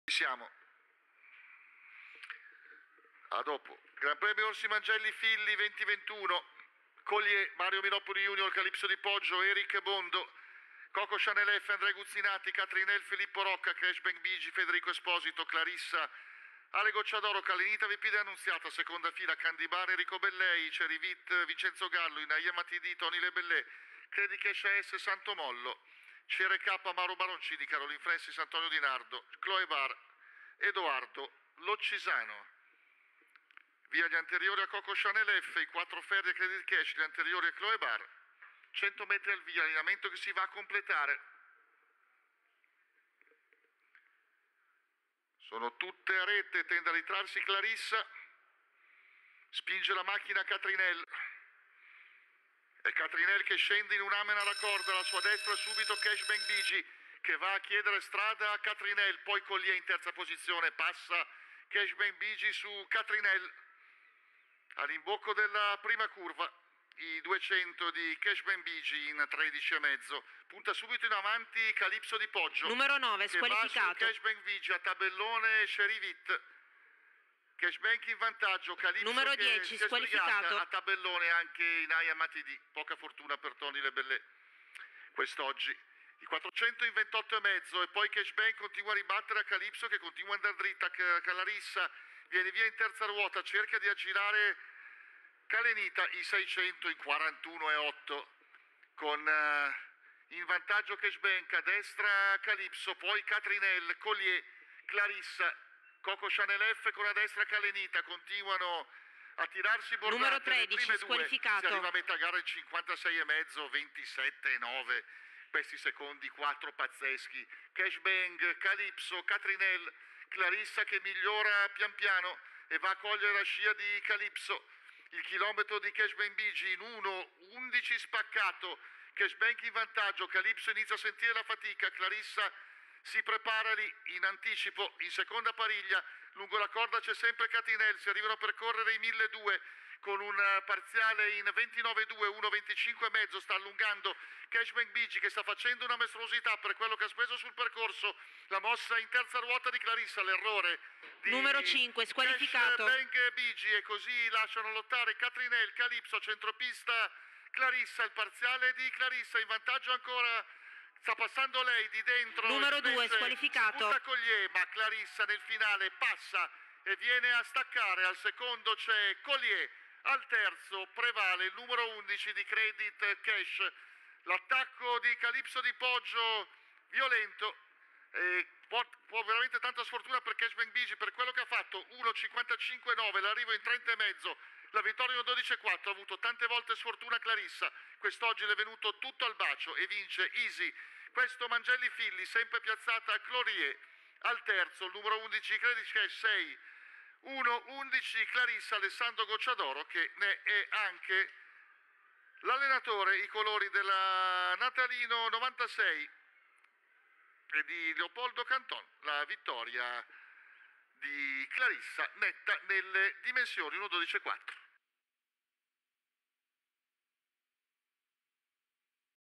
CLARISSA (EXPOLIT CAF) DOMINA IL GRAN PREMIO ORSI MANGELLI FILLY 2021 - IPPODROMO VINOVO, 1/11/2021
ARRIVO ORSI MANGELLI FILLY:
1° 6 CLARISSA - A. GOCCIADORO - 1.12.4 🇮🇹
2° 1 COLLIER - M. MINOPOLI JR - 1.12.8 🇮🇹
3° 11 CREDIT CASH AS - SANTO MOLLO - 1.13.1  🇮🇹
4° 7 CALENITA - V.P. DELL'ANNUNZIATA - 1.13.1  🇮🇹
5° 3 COCO CHANEL EFFE - A. GUZZINATI - 1.13.1  🇮🇹